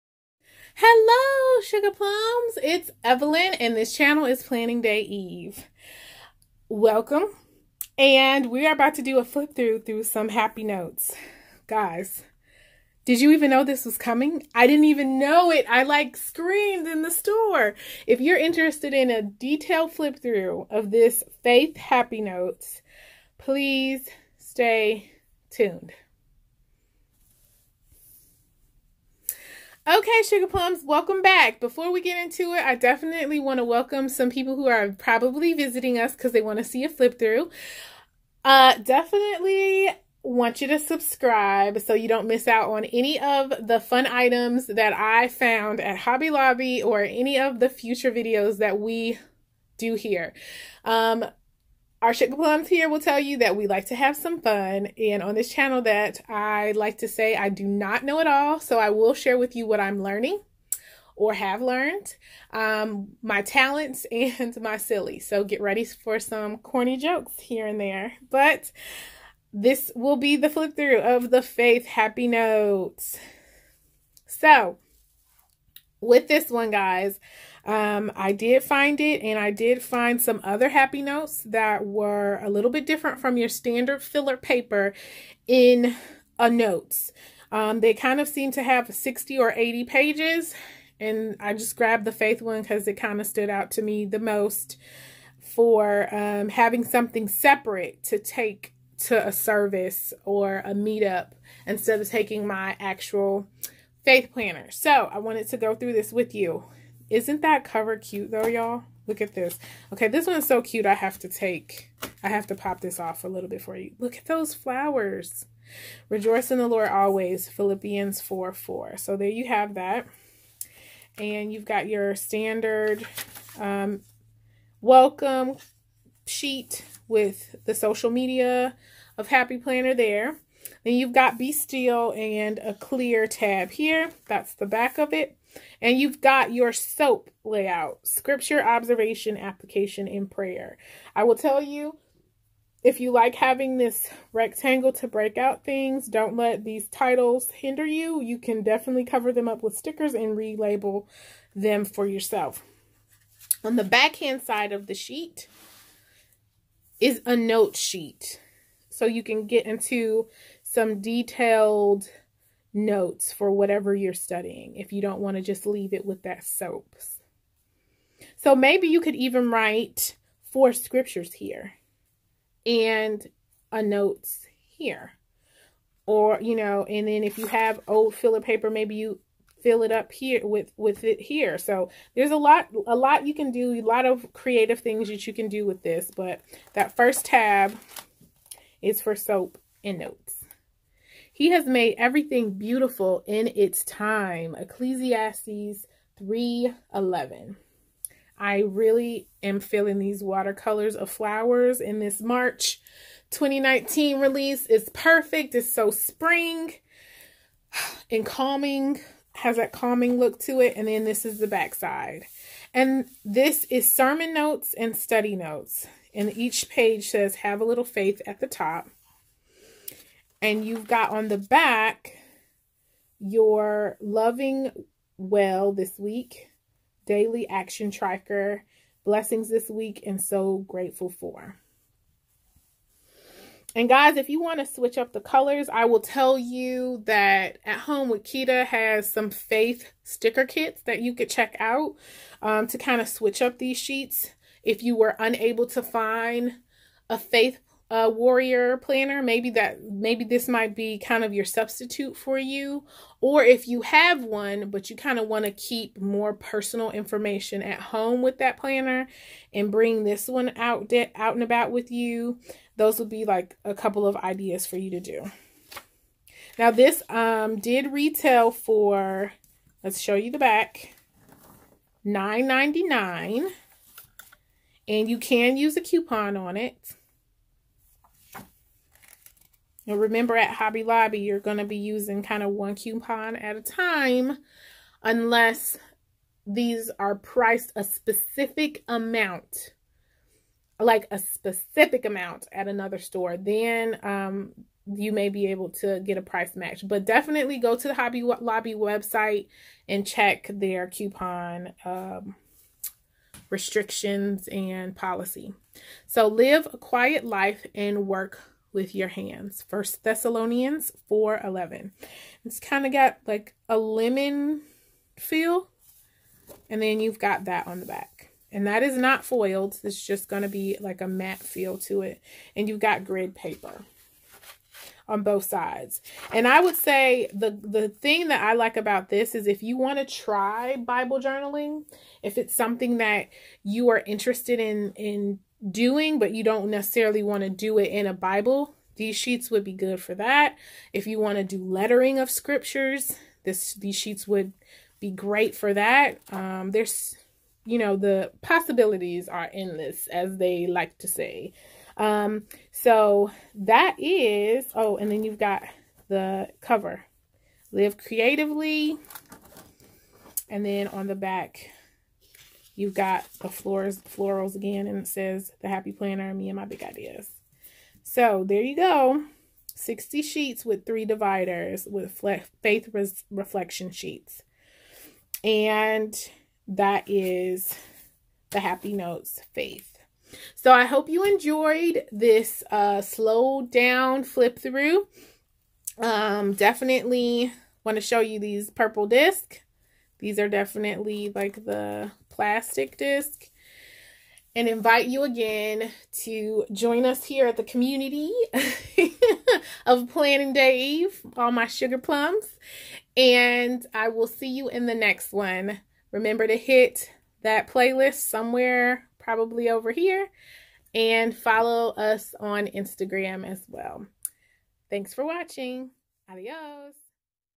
Happy Notes Faith Flip-through| Happy Planner March 2019 Release
This is a detailed flip and first impressions through of Faith Happy Notes: Notebook.

Shopping Tip: I found this item at Hobby Lobby. Please take note that locations initally may receive a small supply however, should be restocked in a month. It will be available on the Me and My Big Ideas Website March 8th.